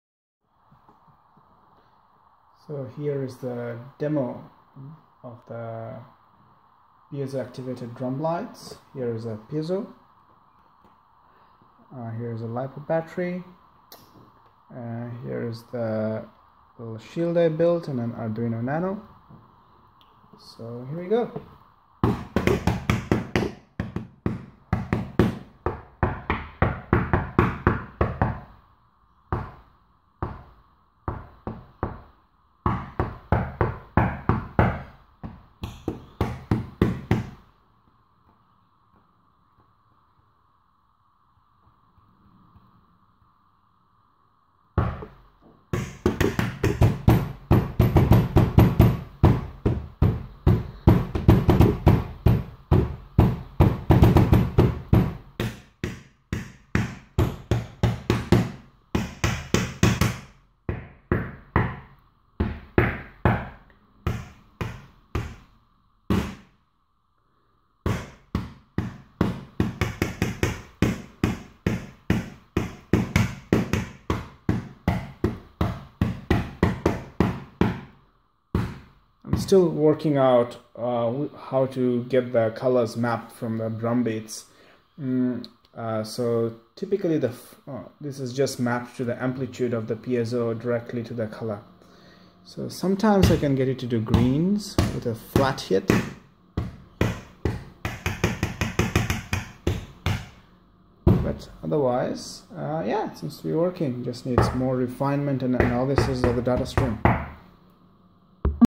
Piezo activated LED strip
Arduino Nano shield with Battery charger and opamp as charge amplifier to capture signals from piezo and turn on RGB LED's